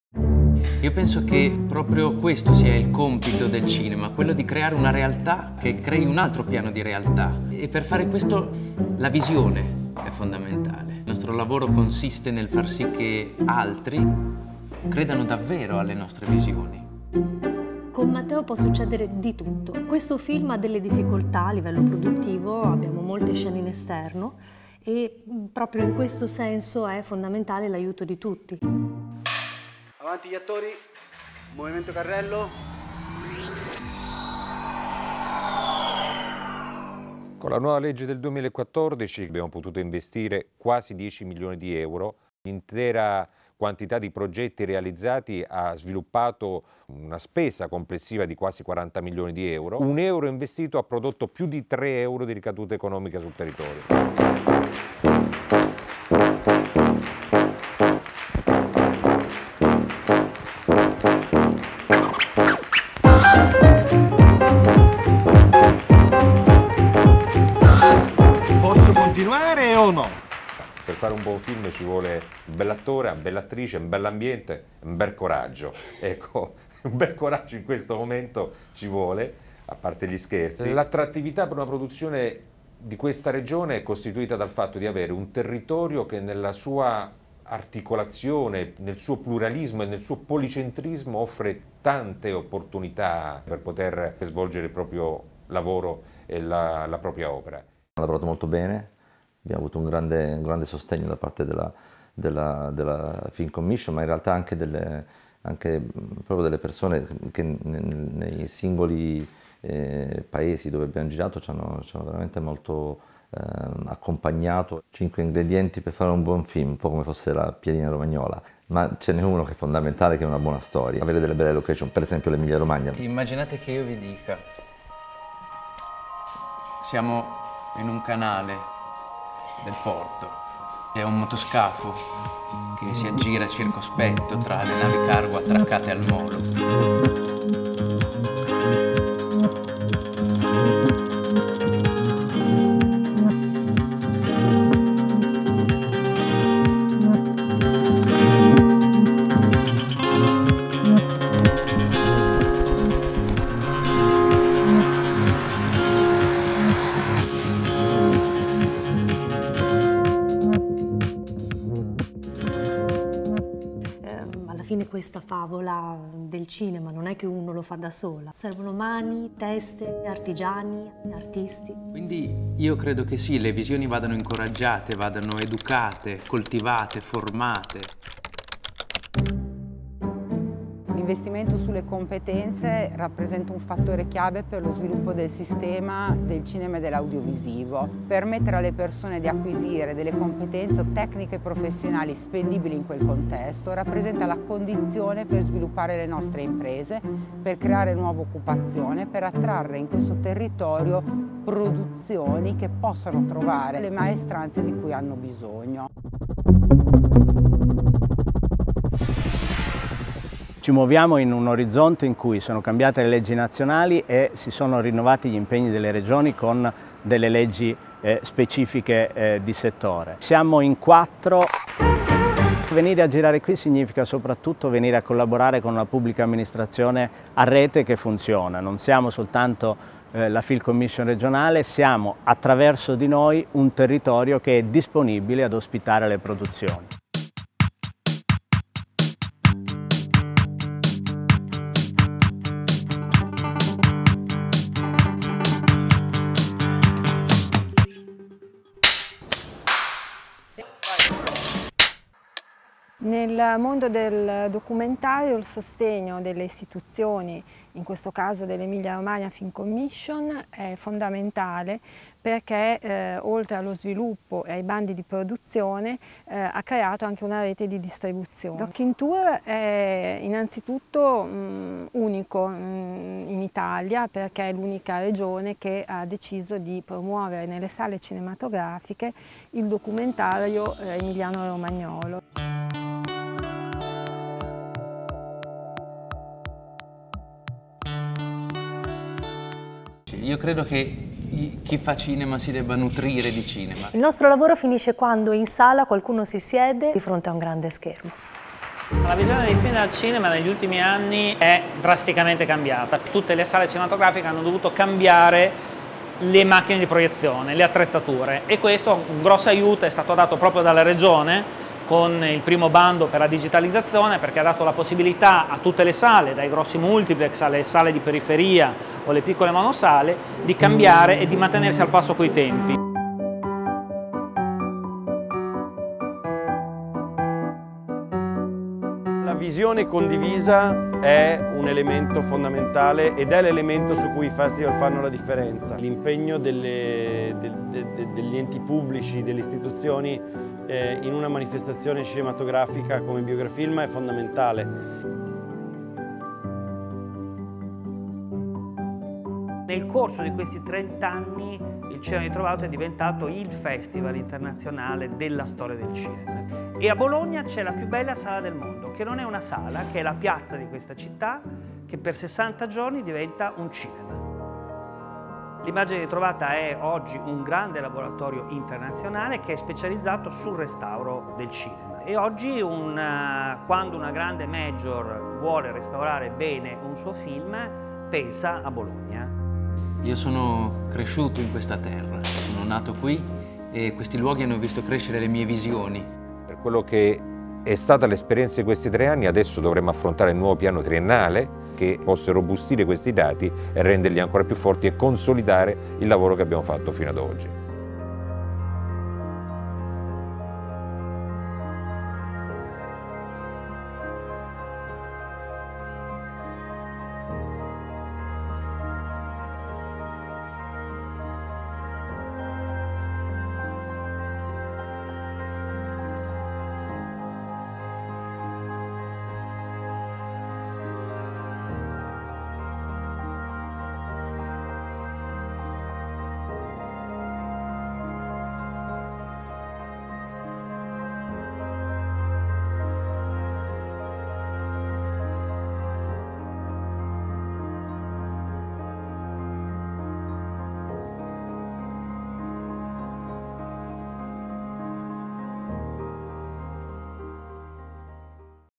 Emilia-Romagna Film Commission si racconta
Accoglienza, location, servizi, rete territoriale. Emilia-Romagna Film Commission incentiva, promuove, sostiene le produzioni cinematografiche e audiovisive, sia italiane sia estere, con l’obiettivo di valorizzare il patrimonio culturale, ambientale e storico del territorio, nonché le risorse umane che vi operano. Ora si racconta in un video, presentando il sistema cinema della Regione Emilia-Romagna, a tre anni dalla Legge 20.